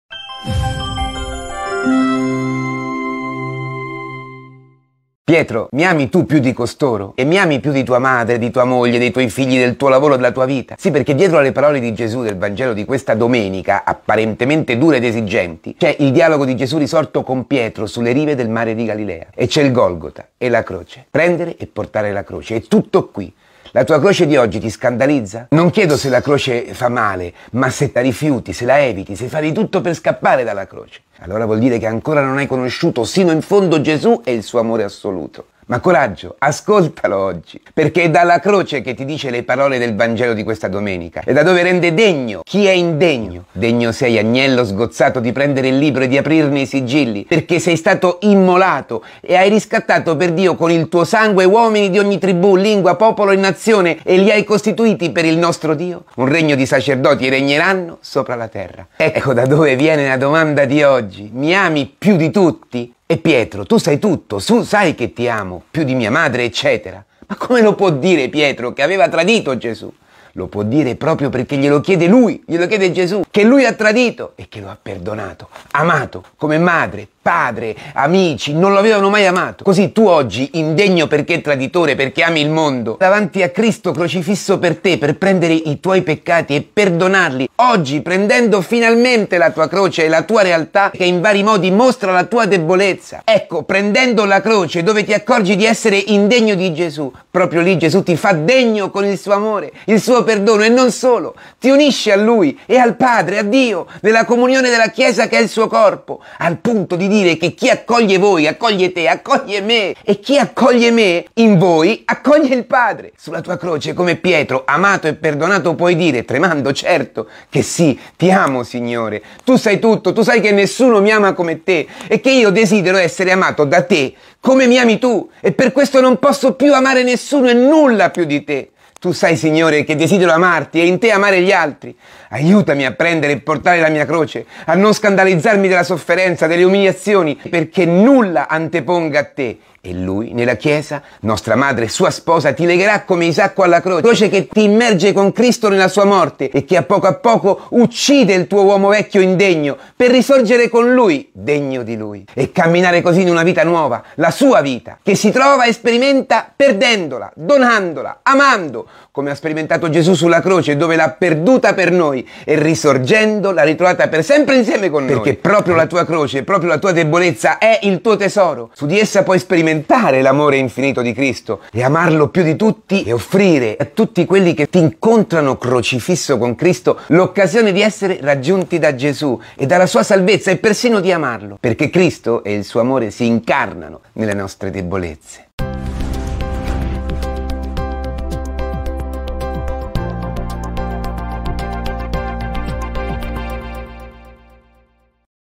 vangelodelladomenica 2 Luglio 2023 - Commento al vangelo di don Antonello Iapicca
XIII DOMENICA DEL TEMPO ORDINARIO. ANNO A
✝️ Commento al brano del Vangelo di:  ✝ Mt 10,37-42
Sulla nostra croce Gesù si dona a noi, ci perdona rendendoci degni di Lui con un amore che nessuno può avere. Per questo prendendo la croce e seguendolo, possiamo amare Gesù più di ogni altra persona, sperimentando che se perdiamo la vita per Lui, la ritroveremo moltiplicata.